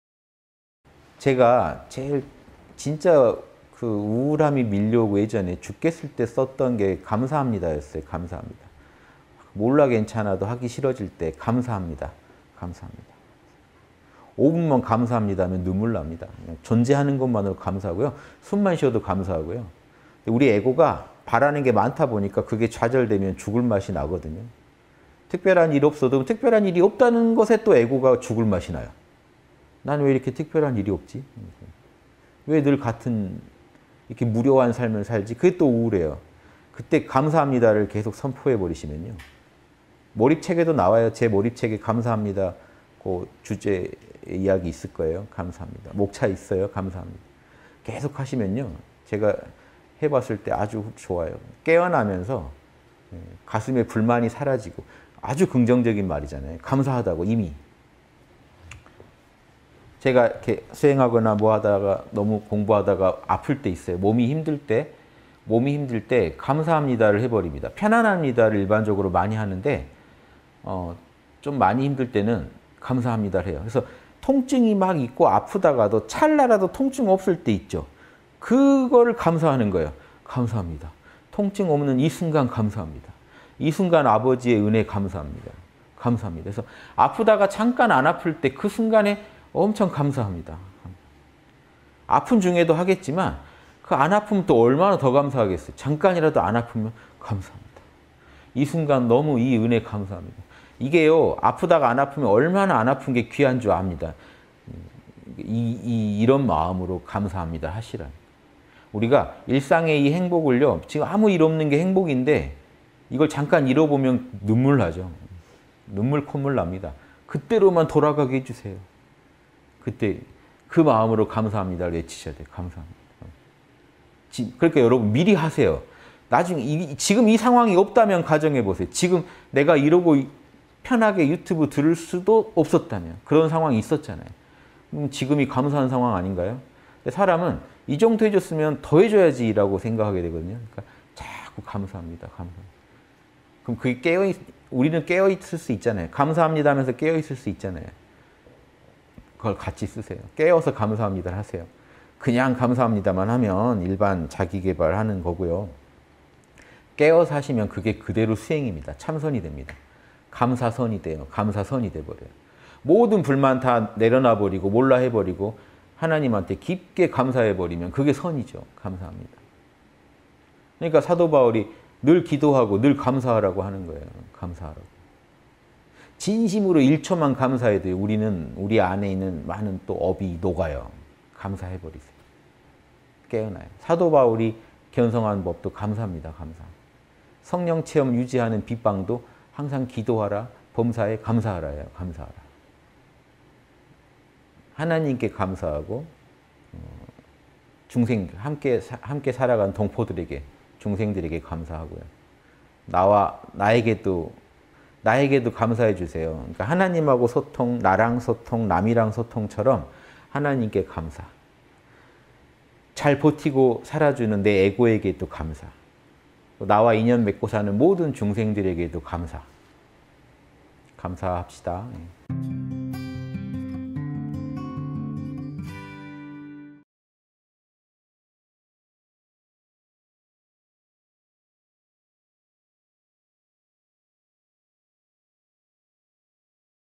[3분 인문학] "감사합니다!"의 효과 _홍익학당
(200815 양심톡톡 토요) 제가 진짜 우울함이 밀려오고 예전에 죽겠을 때,
썼던 게 '감사합니다' 였어요. '감사합니다.'
'몰라 괜찮아'도 하기 싫어질 때,
'감사합니다, 감사합니다.'
5분만 '감사합니다' 하면 눈물 납니다.

존재하는 것만으로도 감사하고요.
숨만 쉬어도 감사하고요.
우리 에고가 바라는 게 많다 보니까
그게 좌절되면 죽을 맛이 나거든요.

특별한 일이 없어도 특별한 일이 없다는 것에
또 에고가 죽을 맛이 나요.
난 왜 이렇게 특별한 일이 없지.
왜 늘 같은 무료한 삶을 살지.
그게 또 우울해요.

그때 '감사합니다'를 계속 선포해버리시면요.
몰입 책에도 나와요.
제 몰입 책에도 '감사합니다' 주제에
이야기 있을 거예요.
'감사합니다' 목차에 있어요. '감사합니다'

계속하시면요. 제가 해봤을 때, 아주 좋아요.
깨어나면서 가슴에 불만이 사라지고
아주 긍정적인 말이잖아요.
감사하다고 이미. 제가 수행하거나 뭐 하다가
너무 공부하다가 아플 때 있어요.
몸이 힘들 때, 몸이 힘들 때,
'감사합니다'를 해버립니다.

'편안합니다'를 일반적으로 많이 하는데,
좀 많이 힘들 때는 '감사합니다'를 해요.
통증이 있고 막 아프다가도
찰나라도 통증 없을 때 있죠.
그걸 감사하는 거예요.
'감사합니다'

통증 없는 이 순간 감사합니다.
이 순간 아버지의 은혜 감사합니다. 감사합니다.
그래서 아프다가 잠깐 안 아플 때,
그 순간에 엄청 감사합니다.
아픈 중에도 하겠지만,
안 아프면 얼마나 더 감사하겠어요.
잠깐이라도 안 아프면, 감사합니다.
이 순간, 이 은혜 너무 감사합니다.
이게요. 아프다가 안 아프면,
얼마나 안 아픈 게 귀한지를 압니다.

이런 마음으로 감사합니다. 하시라고요.
우리가 일상의 행복을요.
아무 일 없는 게 행복인데,
이걸 잠깐 잃어보면 눈물 나죠.
눈물 콧물 납니다.
그때로만 돌아가게 해주세요.
그때 그 마음으로 감사합니다.
외치셔야 해요. 감사합니다.
그러니까 여러분, 미리 하세요.
지금 이 상황이 없다면, 가정해보세요.
지금 내가 이러고 편하게
유튜브 들을 수도 없었다면
그런 상황 있었잖아요.
그러면 지금이 감사한 상황 아닌가요.

사람은 이 정도 해줬으면
더 해줘야지라고 생각하게 되거든요.
그러니까 자꾸 감사합니다. 감사합니다.
우리는 깨어있을 수 있잖아요.
'감사합니다' 하면서 깨어있을 수 있잖아요.
그걸 같이 쓰세요. 깨어서 '감사합니다' 하세요.
그냥 감사합니다만 하면
일반 자기 계발하는 거고요.
깨어서 하시면 그게 그대로 수행입니다.
참선이 됩니다. '감사선(禪)'이 돼요.
'감사선(禪)'이 돼버려요.

모든 불만 다 내려버리고 '몰라' 해버리고
하나님한테 깊게 감사해버리면
그게 선이죠. '감사합니다.'
그러니까 사도 바울이 늘 기도하고
늘 감사하라고 하는 거예요. '감사합니다.'

진심으로 1초만 감사해도 우리는
우리 안에 있는 많은 업이 녹아요.
감사해버리세요. 깨어나요.
사도 바울이 견성한 법도 '감사합니다, 감사합니다.'
성령체험 유지하는 비방도 '항상 기도하라.
범사에 감사하라'예요. 감사하라.

하나님께 감사하고,
함께 살아가는 동포들에게,
중생들에게 감사하고요.

나에게도 감사해주세요. 하나님하고 소통,
나랑 소통, 남이랑 소통처럼
하나님께 감사.
잘 버티고 살아주는 내 에고에게도 감사.
나와 인연 맺고 사는 모든 중생들에게도 감사.
감사합시다.